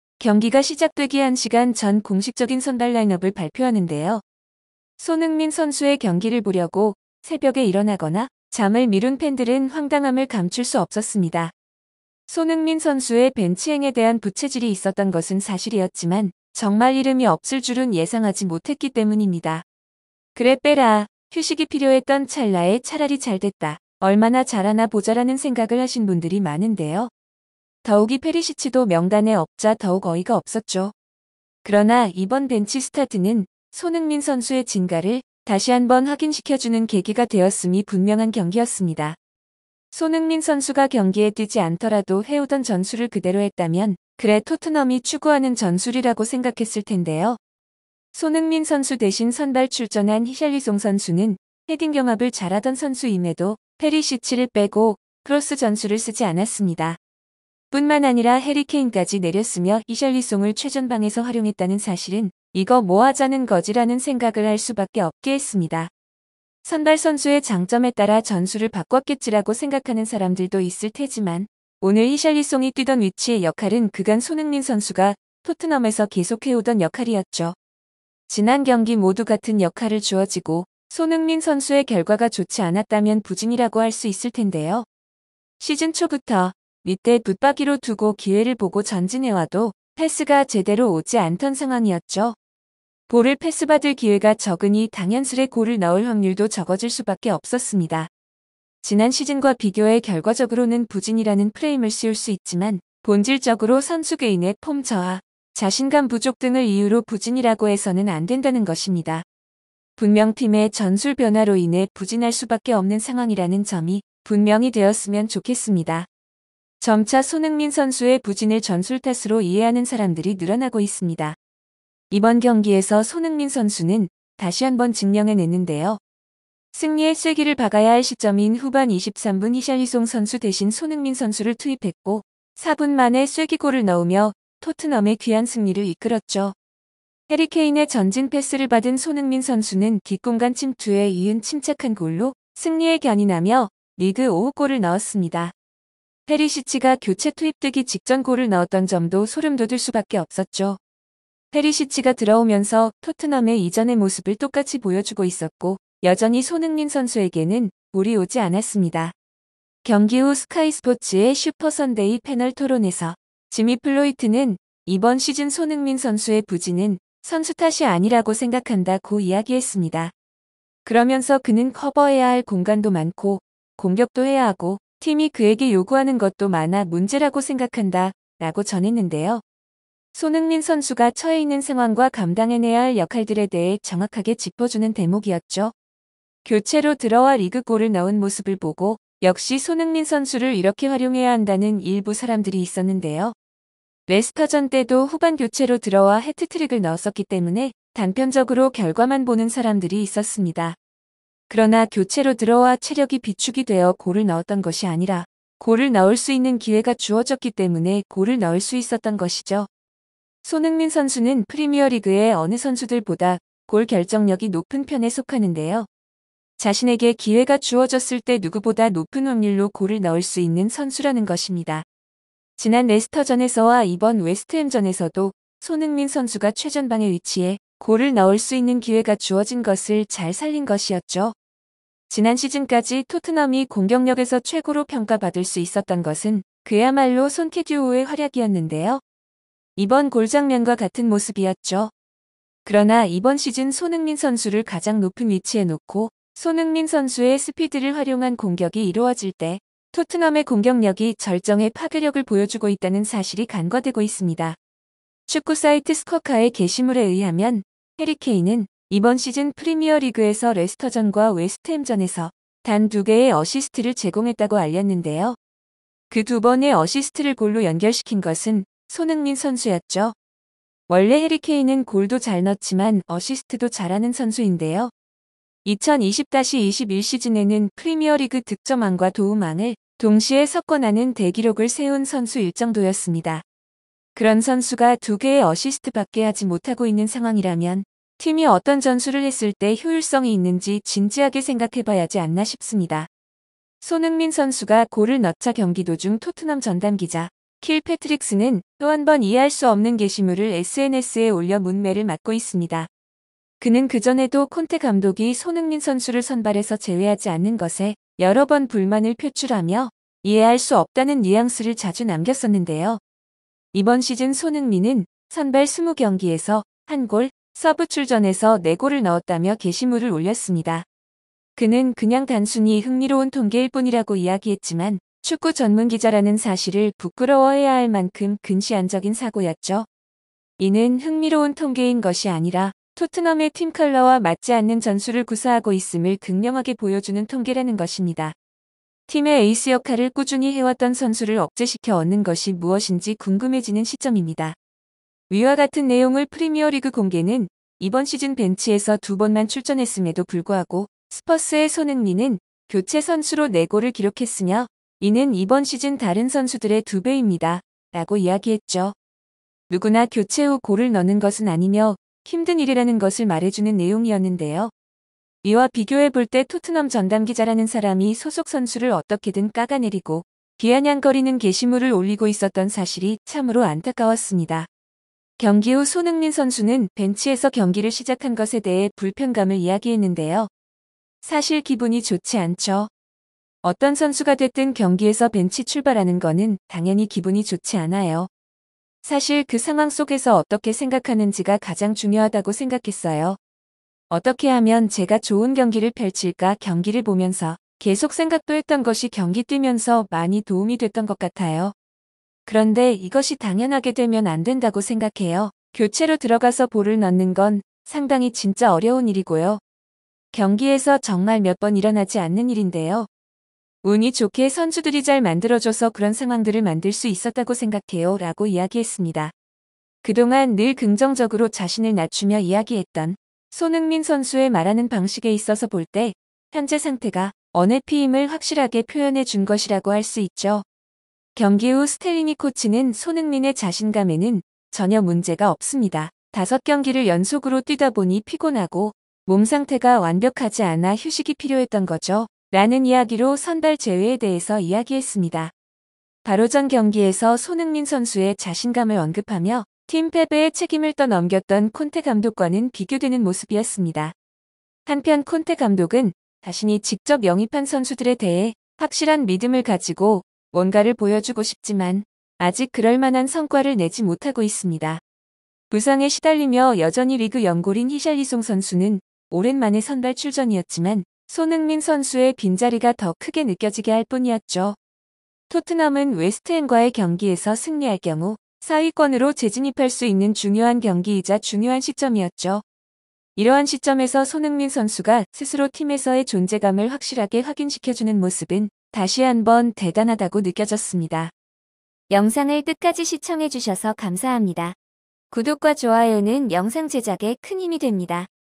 "손흥민 복귀골 저질렀다;;" 교체 투입 5분만에 '0.3초' 반박자 기습쇄도 7명 역동작;; '원더골' 작렬 BBC해설 경악한 진짜 이유
"손흥민 골보고 무슨생각 했냐고요..??" 경기직후 영국 중계진 한마디에 충격 해리케인마저 죽이는 토트넘 전술에 초비상